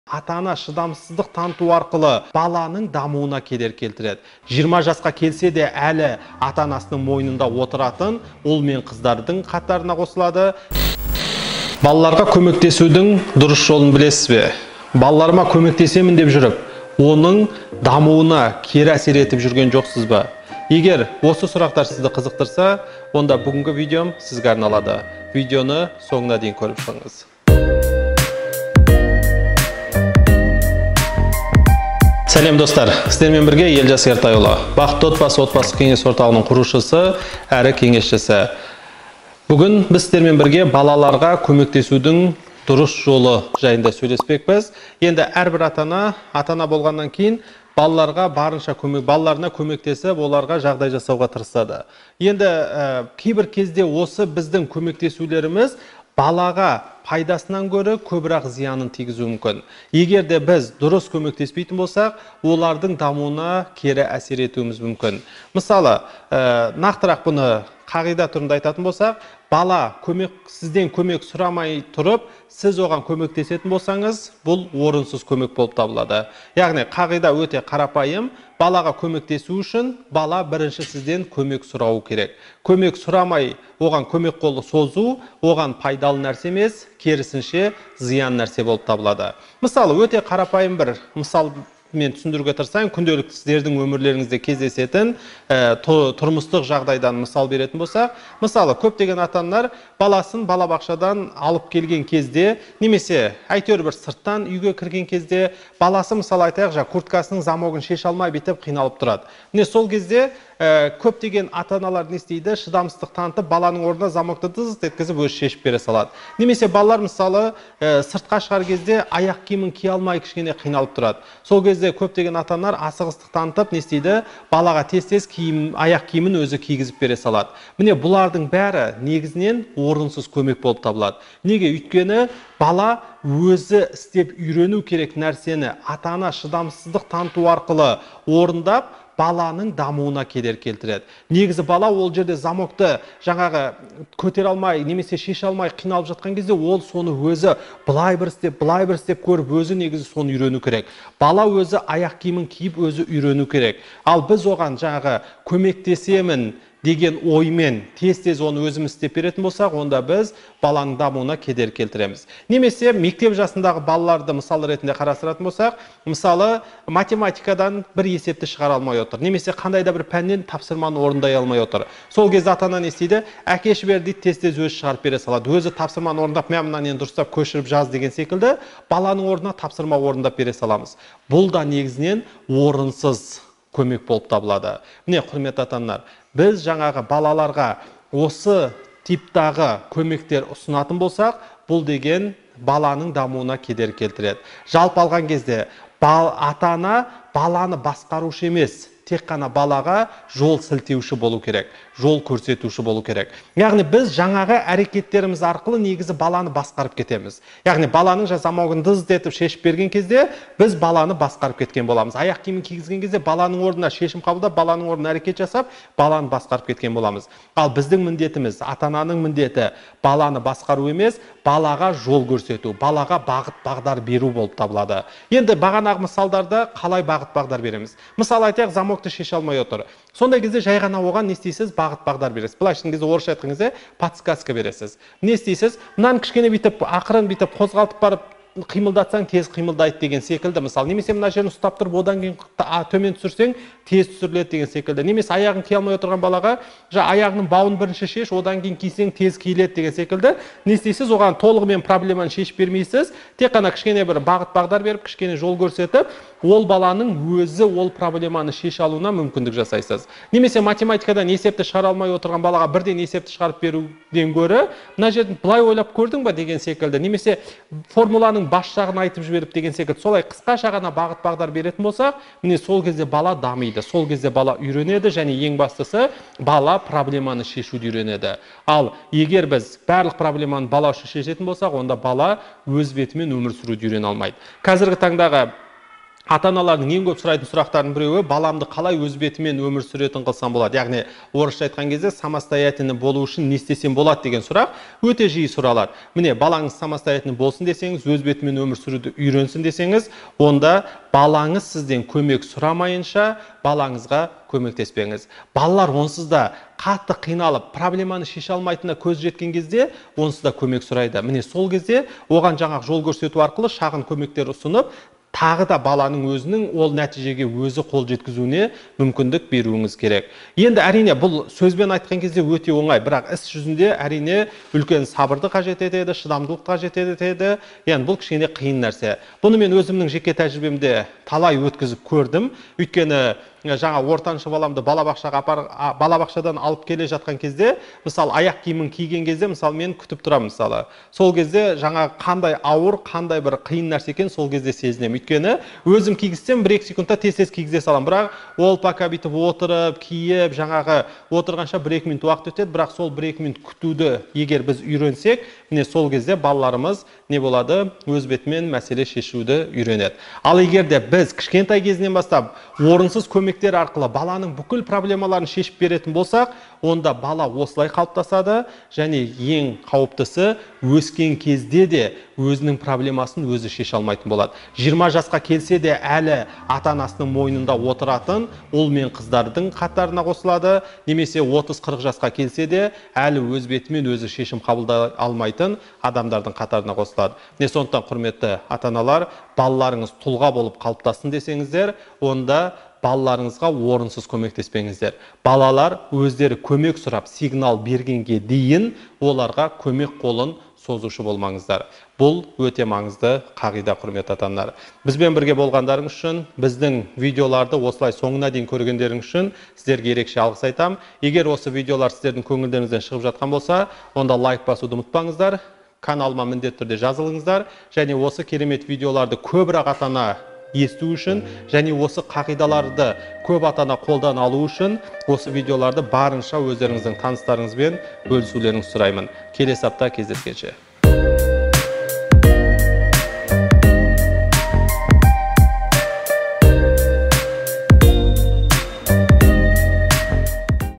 Балаңызға көмектеспеңіз.
Ата-ана шыдамсыздық таныту арқылы баланың дамуына кедергі келтіреді.
20 жасқа келсе де әлі ата-анасының мойнында отыратын ұл мен қыздардың қатарына қосылады.
БАЛАЛАРҒА КӨМЕКТЕСУДІҢ ДҰРЫС ЖОЛЫН БІЛЕСІЗ БЕ?
Балаларыма көмектесемін деп жүріп, оныңдамуына кері әсер етіп жүрген жоқсыз ба? Егер осы сұрақтар сізді қызықтырса, онда бүгінгі видео сізге арналады.